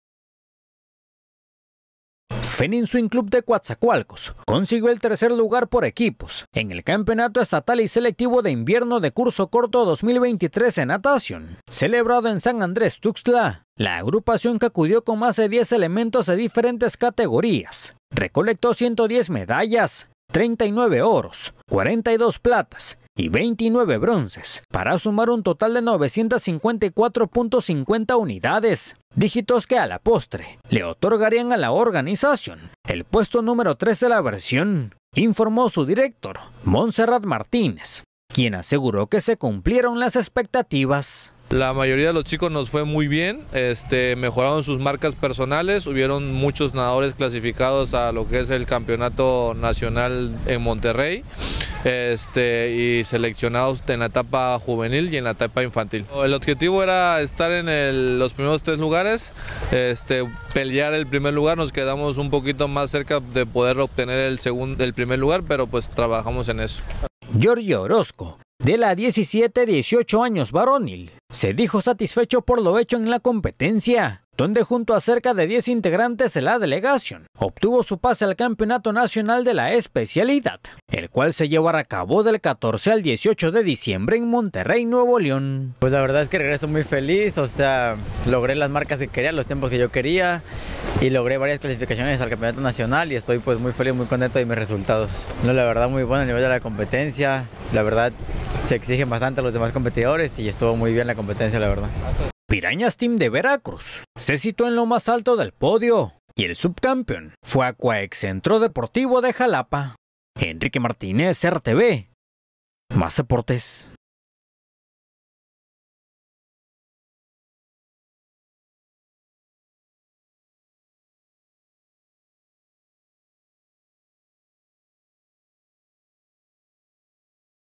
FENIX CAMPEON INVIERNO NATACIÓN
El club Fénix Swin de Coatzacoalcos, obtuvo tercer lugar en campeonato estatal y selectivo de invierno de curso corto 2023 de natación.
Reportero: Enrique Martínez